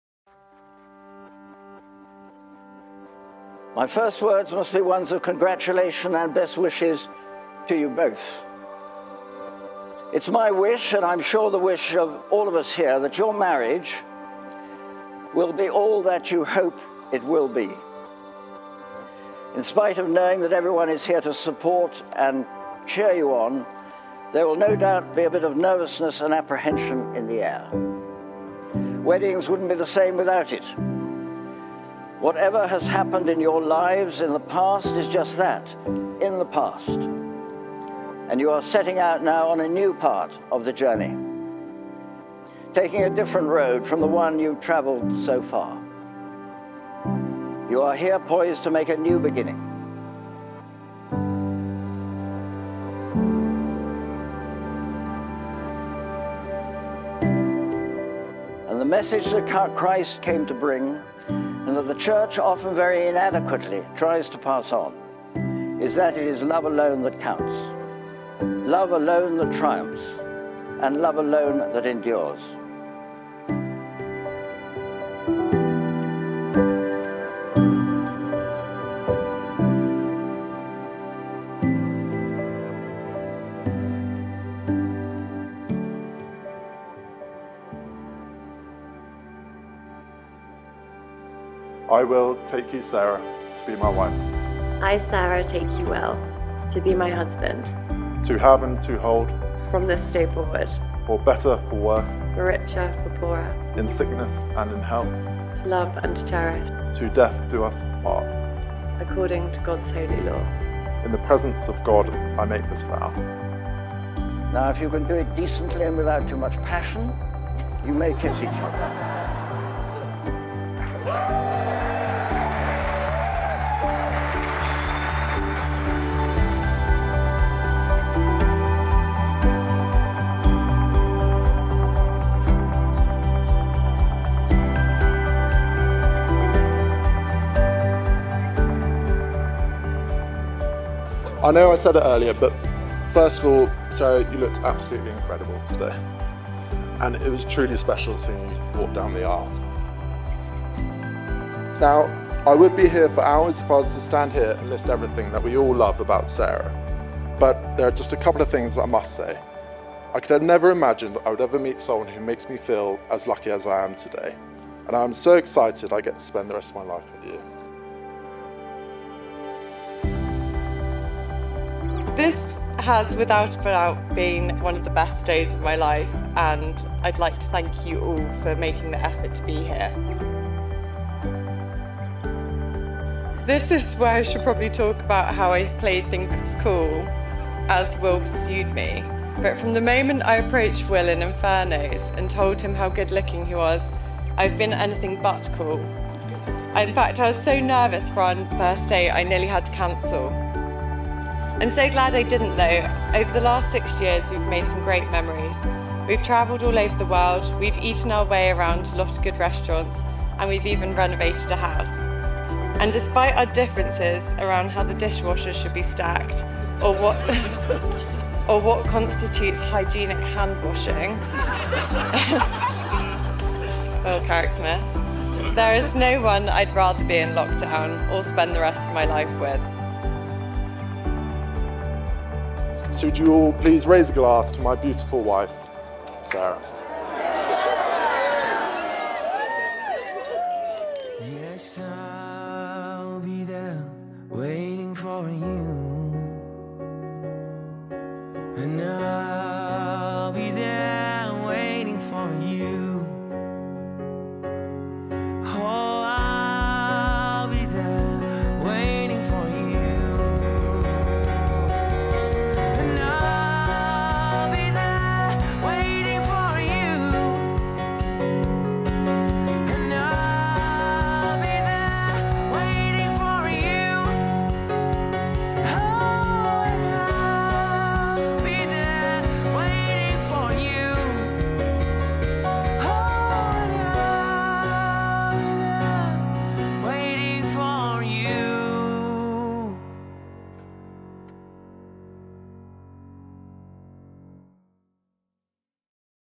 A Warmwell House Wedding
‘it’s absolutely incredible, wow!!! Thank you SO much, that has bought back so many happy memories and I love how you incorporated the song at the end, absolute perfection! Thank you!!’

Situated amongst the rolling hills of Dorset (made famous in the novels of Thomas Hardy), Warmwell House is a true gem of a venue. Dating back to the 17th century, its Jacobean style architecture imbues it with a sense of history and grandeur that compliments any wedding day.

For their Warmwell House Wedding, Sarah & Will decided to both get ready on site - Sarah in the manor house itself, and Will at a separate house within the grounds. The manor house was the perfect spot for bridal prep, the lavish rooms combined with some lovely natural light coming through the windows allowed me to capture some great shots of Sarah and the bridal party getting ready. I chose to overlay parts of the priest’s ceremony address to start the film - quite often this is tricky as the priest will tend to stick too closely to scripture - but in this case the words seemed authentic and relevant enough to provide with a great opening to the film.

Holy Trinity Church, located on the grounds of Warmwell house, was the backdrop for the ceremony. A surprisingly large, bright and spacious church (fortunately so as Will is 6’8” tall!), I was able to get some great angles of both Sarah & Will as they exchanged their vows to one another.

The reception was held on the well-kept lawns of Warmwell House, which except for a short, unseasonal rain shower was the perfect location. Both Sarah & Will did speeches, allowing me to provide both narrative angles when selecting the audio clips to use. This always makes for a more balanced film from a story perspective, so my plea to all future brides is to do a speech themselves! They also both had some heartfelt words to say about one another, how they met and their excitement for their future together. Speeches like this make my job so much easier - they provide me with the perfect audio to overlay over the portrait session, and really further the emotional impact of the film.

To end this Warmwell House Wedding film I used some pre-recorded audio from the one and only Michael G Moore - Piano Animal - who perfromed the couples first dance on the wedding day. It seemed only right to use this music over the clips of Sarah & Will’s first dance, and it made the perfect segue way into the party.

Key Moments
00:00 - Bridal Prep
00:42 - Groom Prep
01:00 - First Look
01:23 - Pre ceremony
01:33 - Vows
02:10 - Confetti
02:25 - Couple Portraits
02:50 - Reception
04:00 - First Dance